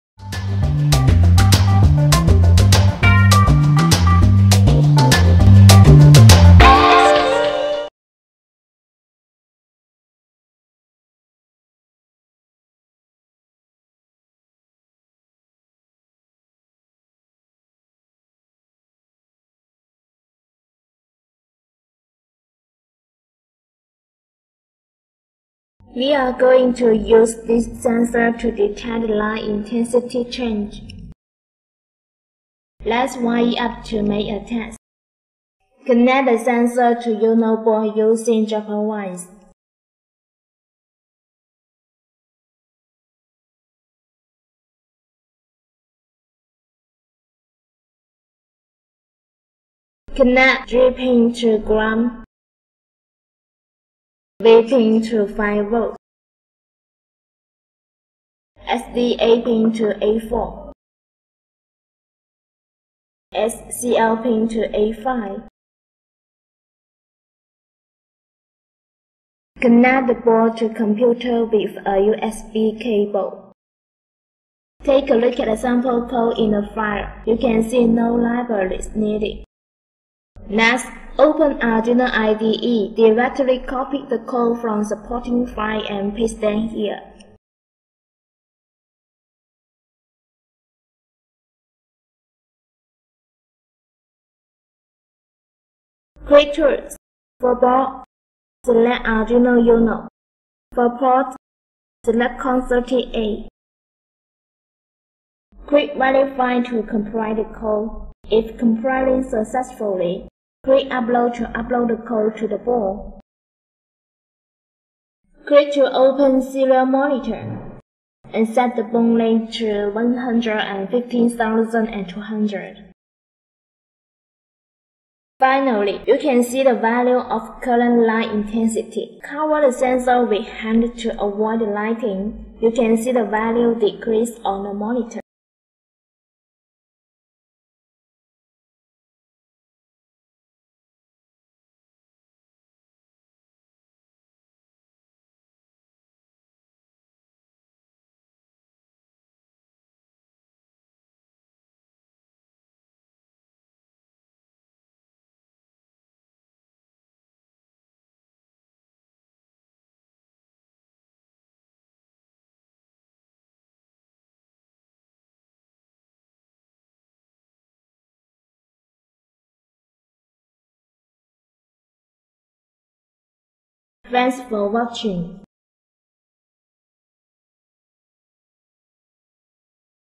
KS0278 Keyestudio BH1750FVI Digital Light Intensity Module (Black and Environmental Friendly)
You can use this sensor to read the light intensity change shown on the serial  monitor.